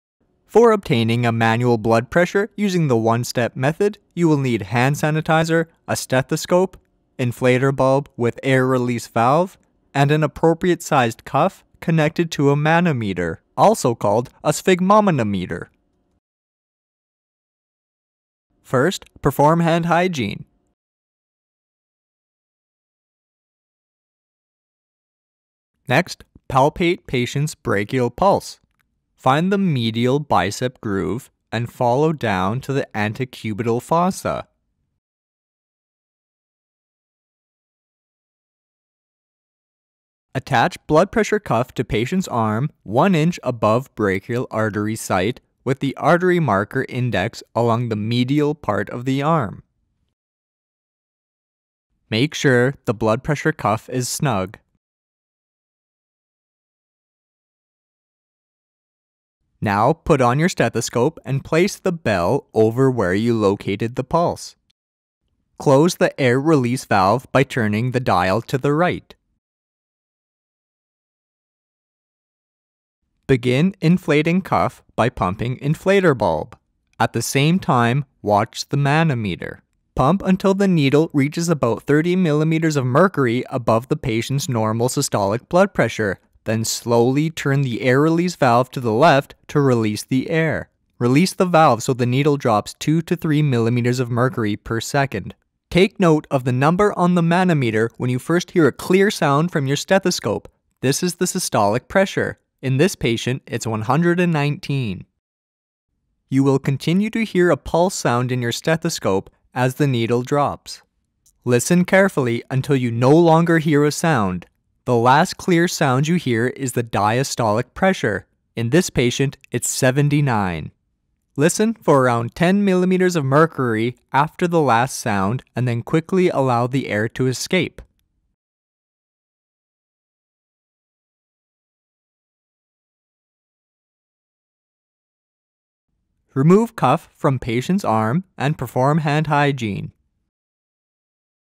Manual Blood Pressure One Step Method
How to do a manual blood pressure using the one step method.

Note: generally the numbers for diastolic and systolic numbers have to be even when using simple clinical tools such as the manual blood pressure machine. (in this video, the systolic should be 120 and diastolic 80). This is because the manual tools are generally calibrated to be accurate within 2mmhg.

Automatic blood pressure machines are usually calibrated to be more accurate to within 1mmhg (and display odd number readings), but if you suspect anything to be wrong, incorrect or need to confirm an unusually high/low BP from the automatic machines, then proceed to confirm with a manual blood pressure reading.

Your measurements are only as accurate as the method of collecting, which is why in the ICU and ER, an arterial line is used to obtain blood pressure readings in unstable/critical patients to obtain more accurate numbers and can display odd number readings.